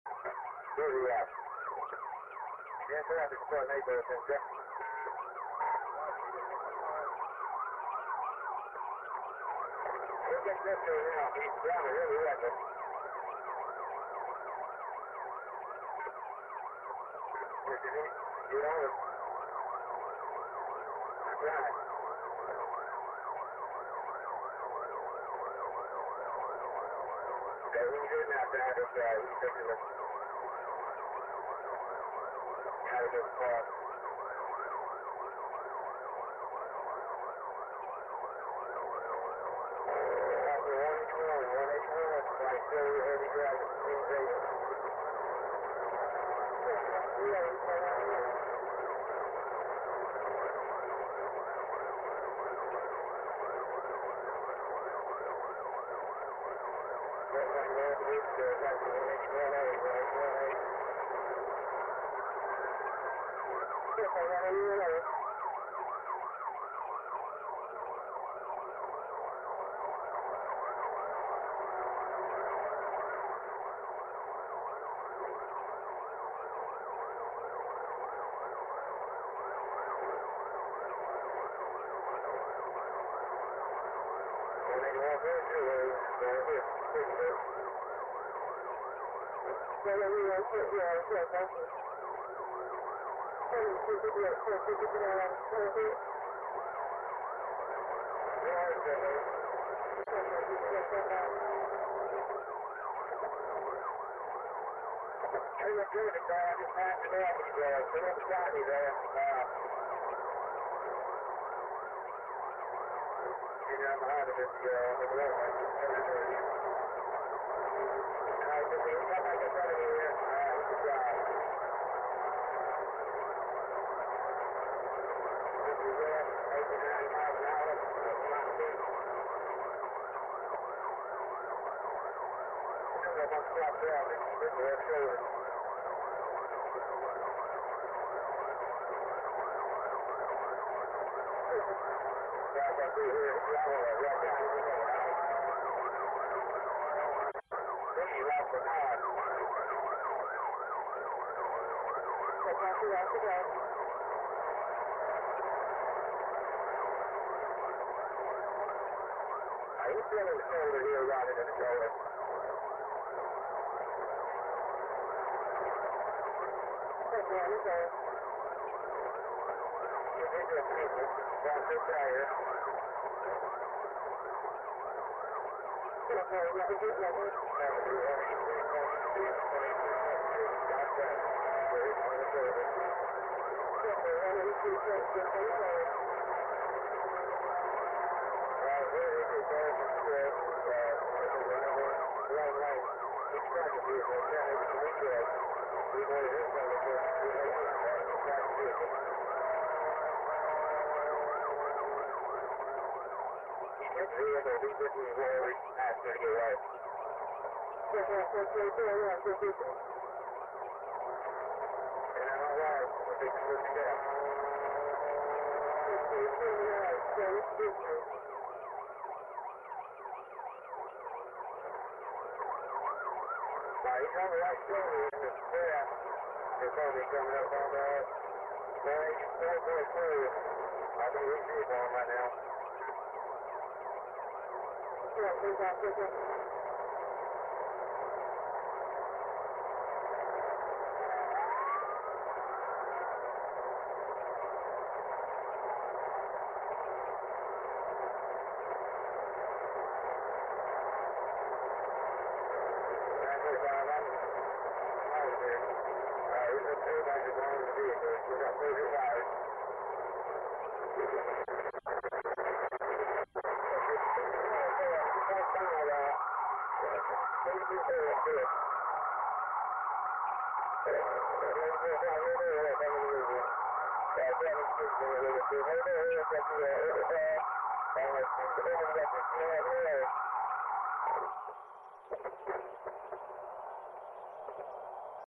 THP Chase 1
Video released today by the Tennessee Highway Patrol from the dashcam of the trooper who crashed in the Feb 24 high speed chase on I-24 & 75.